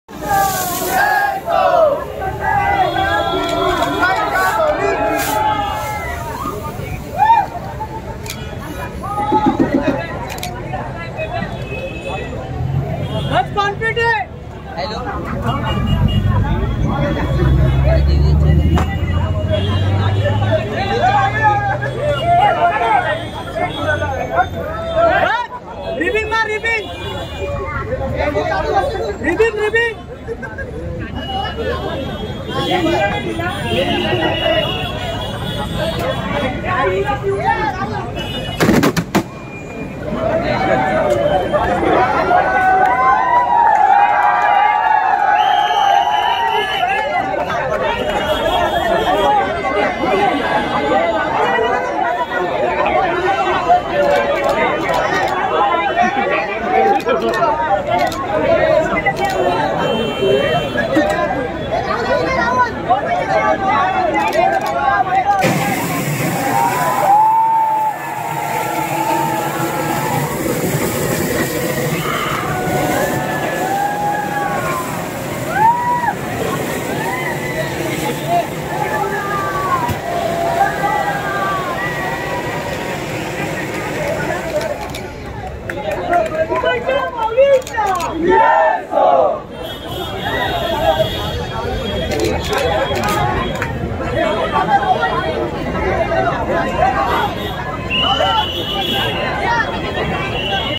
Mumbaichi Mauli Aagman Sohala 2021 at Vijay Khatu Workshop Parel | मुंबईची माऊली आगमन | Devi Aagman
Mumbaichi Mauli Aagman Sohala 2021| मुंबईची माऊली आगमन 2021| Devi Aagman 2021

Video by :-  sujitk.bhardwaj